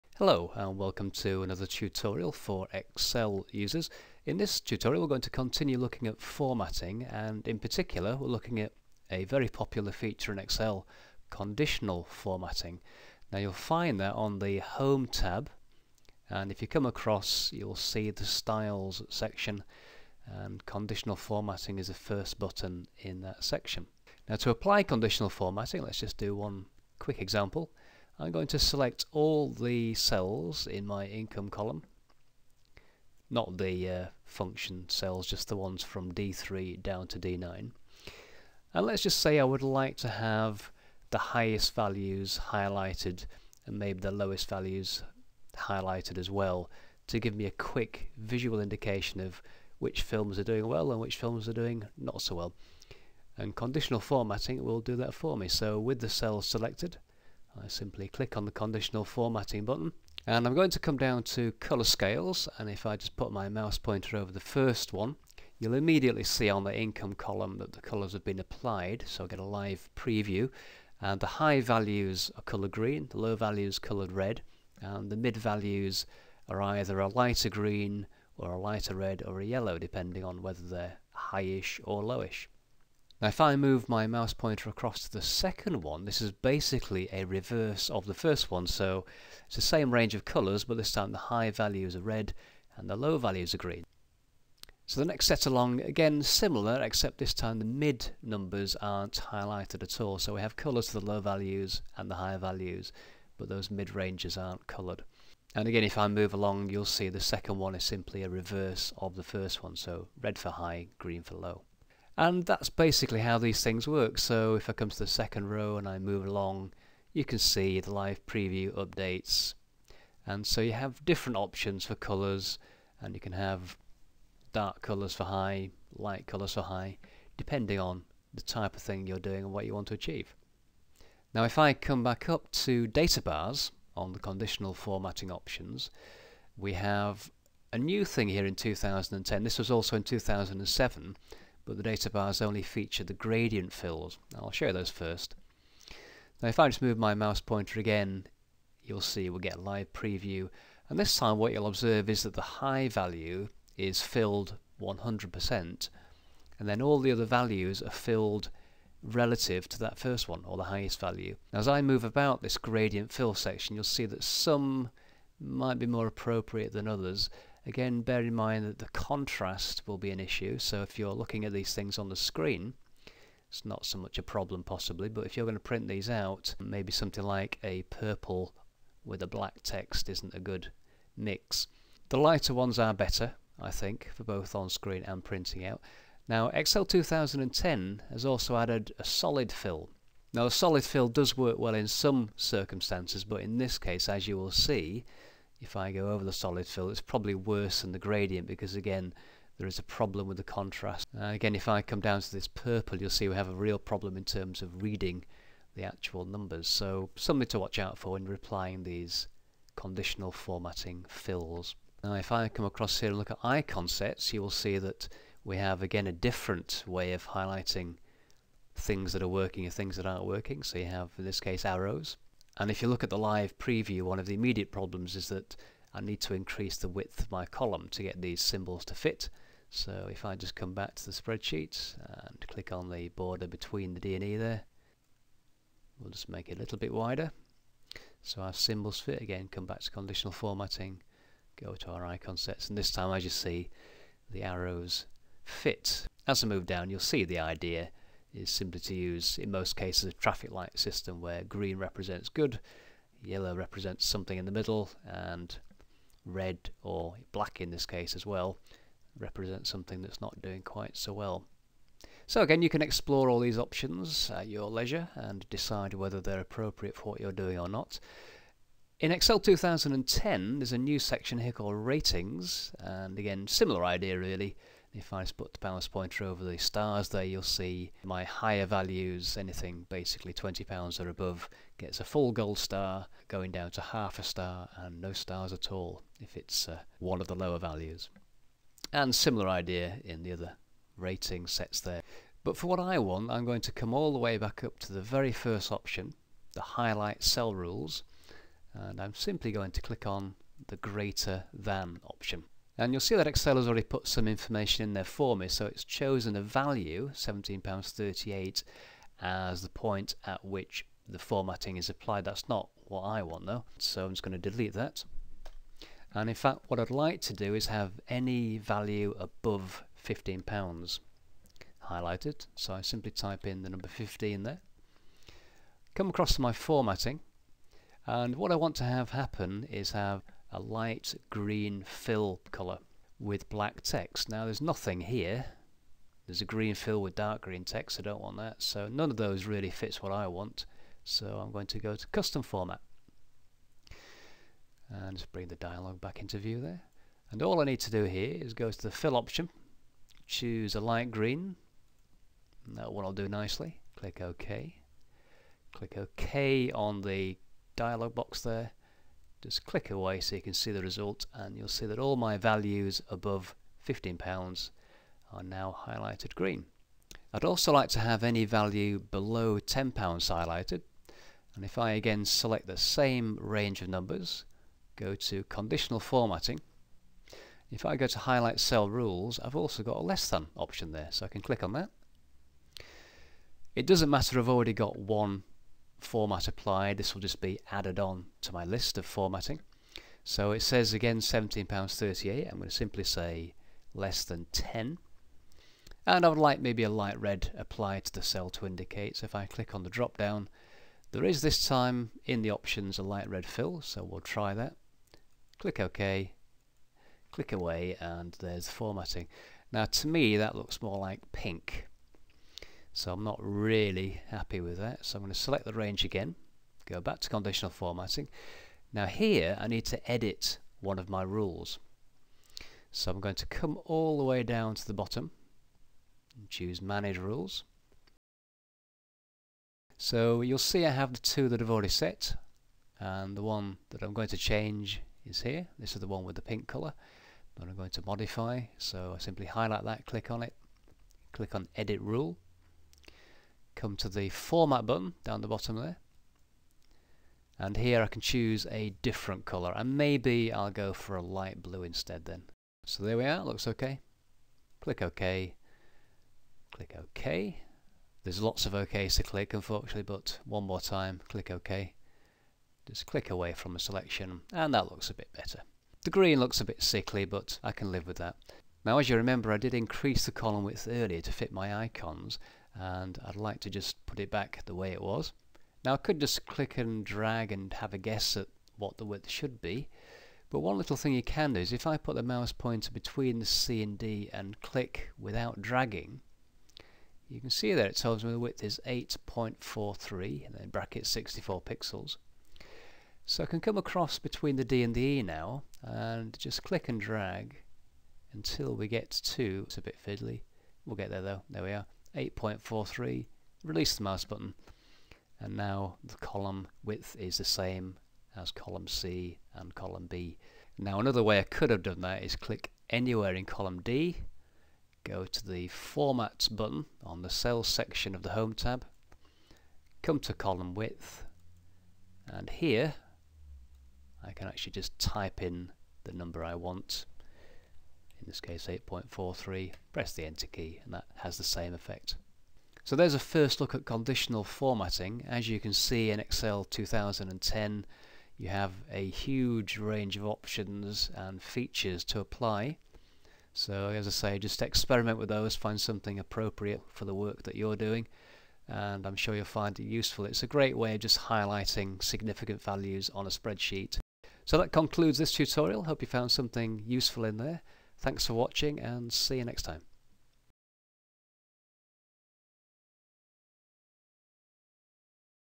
Excel 2010 Tutorial For Beginners #8 - Conditional Formatting Pt.1 (Microsoft Excel)
This updated Excel 2010 tutorial shows how to use the Conditional Format tools and a few of the new features in Excel 2010.  More tutorials to follow.